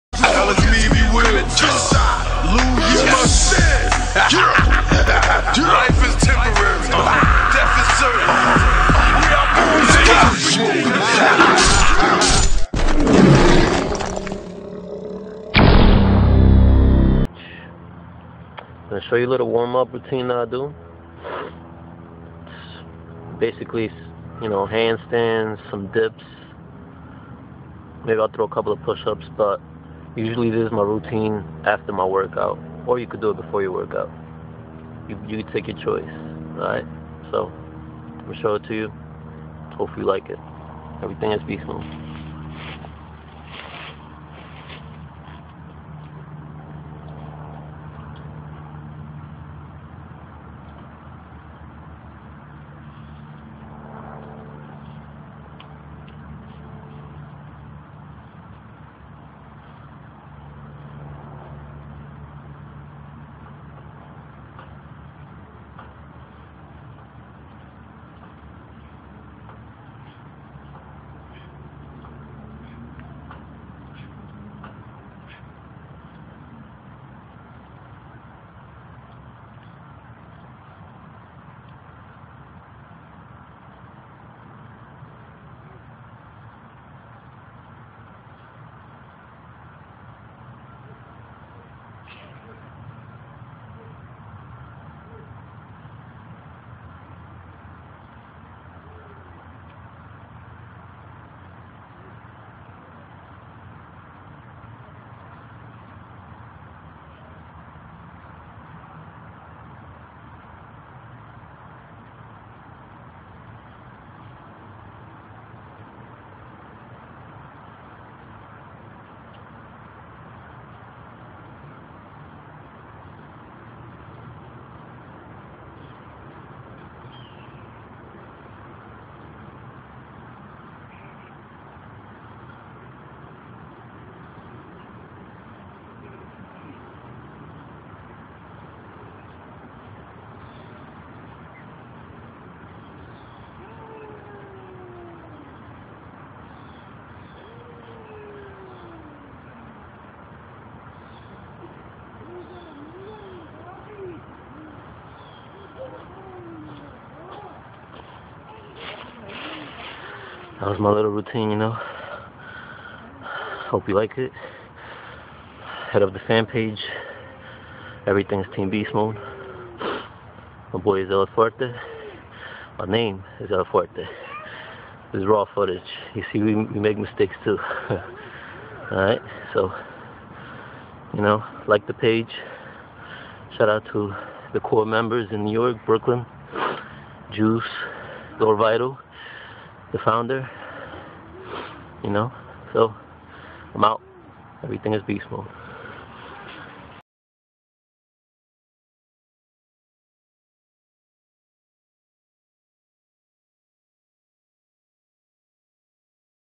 Everything Is Beastmode tv / el "Fuerte" New Jersey division
Everything Is Beastmode - presents: el "Fuerte" one of our members in the Jersey Division. This is one of his quick routines after his regular workout. 5sec. Handstand hold full lockout, 5 russian dips, 10 dips strict form, 10sec. L-sit hold, 15 pushups strict form. All in one set no dropping from the bars.